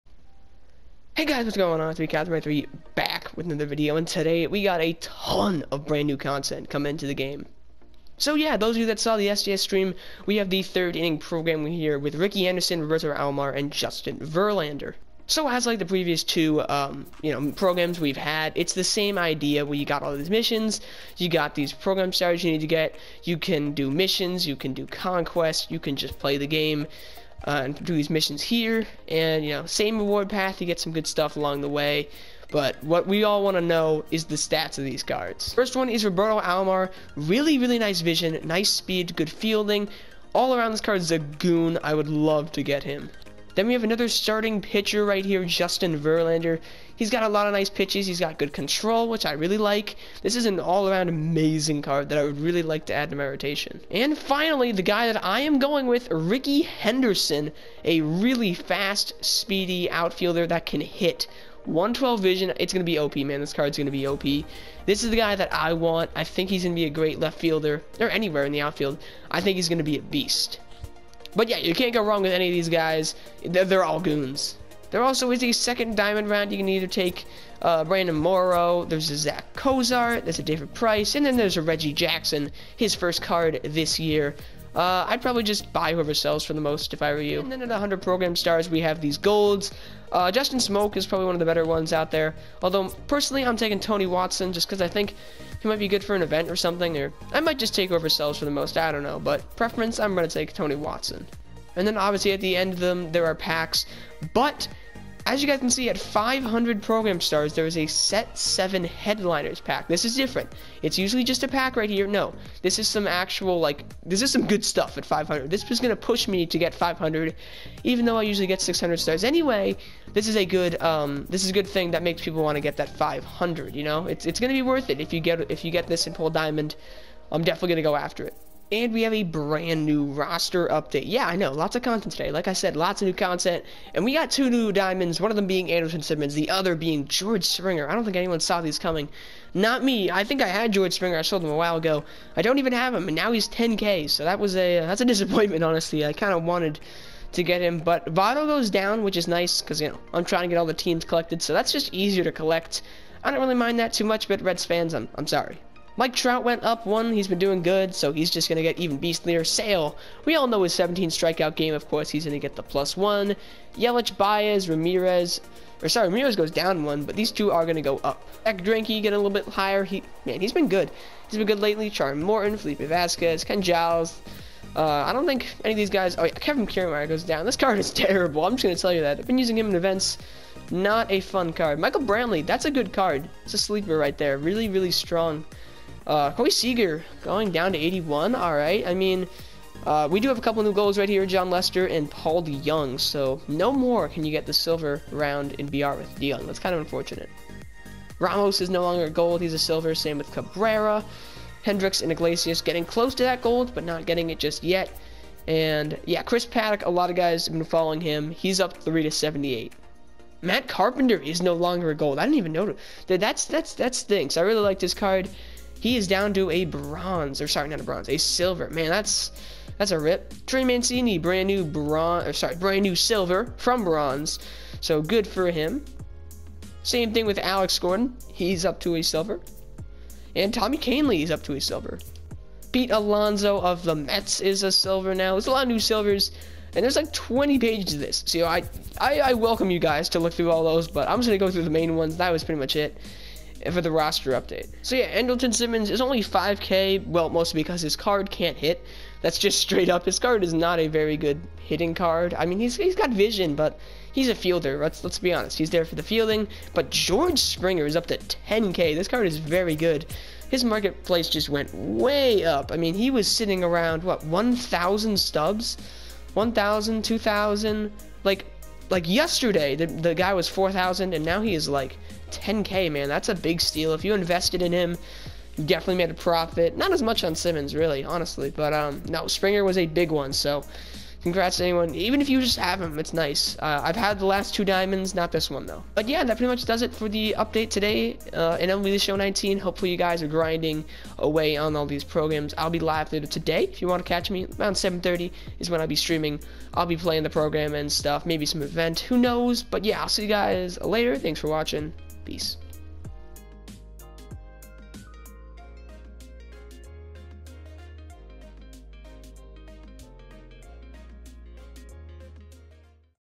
Springer And Simmons Are Diamonds! - MLB 19 News
In this video I go over all the content added today in MLB The Show 19.